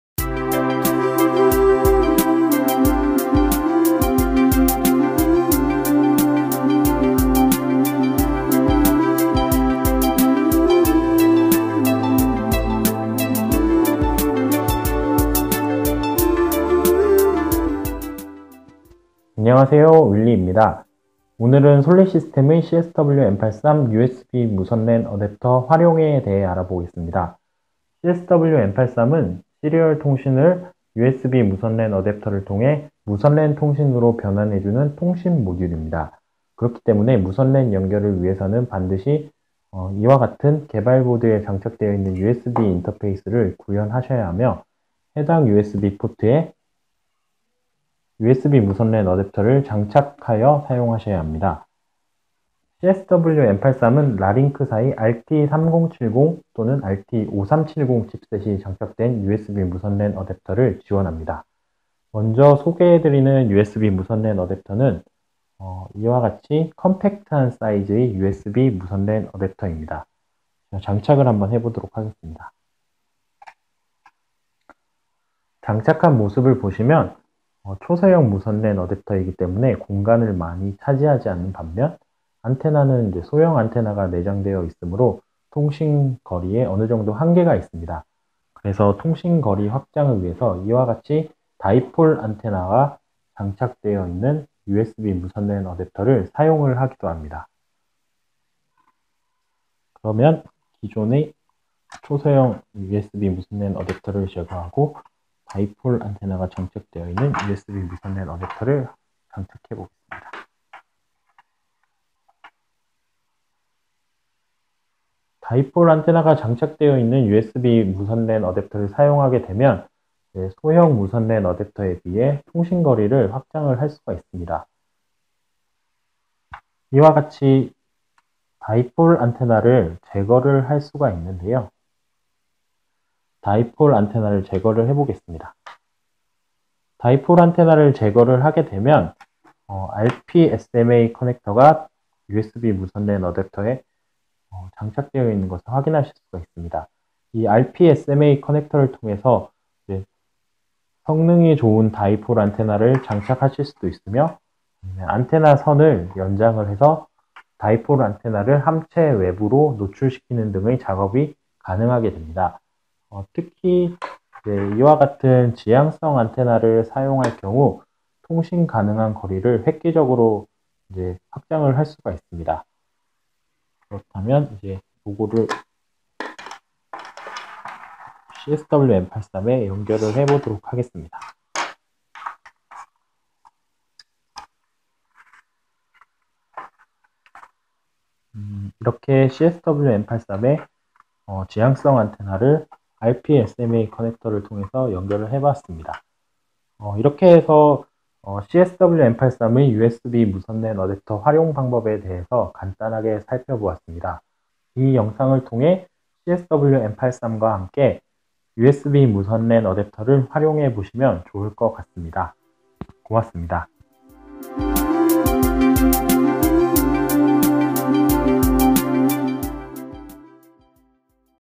CSW M83의 USB 무선랜 어댑터 활용
솔내시스템의 CSW-M83은 시리얼 장비를 무선랜(WiFi)으로 제어 및 관리할 수 있도록 해주는 시리얼 무선랜 모듈(Embedded Serial to WiFi Module)입니다. 특히 CSW-M83은 USB 무선랜 어댑터를 외부에 장착하는 형태로 설계되어 있어 RF 승인을 별도로 고려할 필요가 없습니다.
이 동영상에서는 CSW-M83에 장착할 수 있는 다양한 형태의 USB 무선랜 어댑터에 활용에 대해서 소개합니다.

 IEEE 802.11b/g 무선 표준에 맞게 설계된 CSW-M83을 사용자 장비에 시리얼(UART)로 연결하면 소프트웨어 변경 없이 간단히 무선랜 네트워크에 연결할 수 있습니다. 따라서 기존 사용자 장비의 통신거리를 확장할 수 있을 뿐만 아니라 유선 작업이 어려운 환경에서도 편리하게 네트워크를 구성할 수 있습니다.

CSW-M83은 무선랜 통신이 필요한 각종 시리얼 장비에 사용 가능하며, 특히 POS 단말기, 서바이벌 게임, 차량진단 장비 등 다양한 응용분야에 활용할 수 있습니다.

Sollae Systems Co.,Ltd.